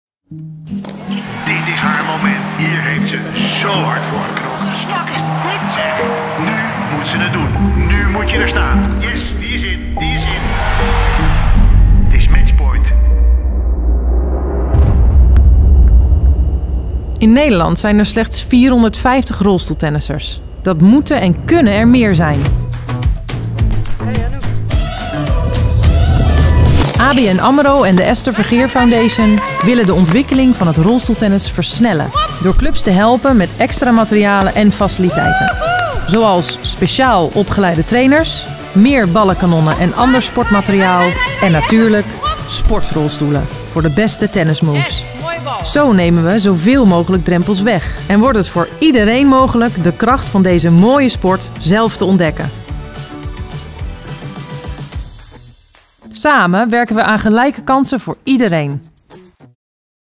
Tennisclubs worden Club van Morgen
Ruim 1 miljoen tennissers en maar 450 rolstoeltennissers. Dat moeten en kunnen er meer worden.

Samen met de Esther Vergeer Foundation maken we tennisclubs door heel Nederland rolstoelvriendelijk, zodat iedereen de kracht van sport zelf kan ervaren.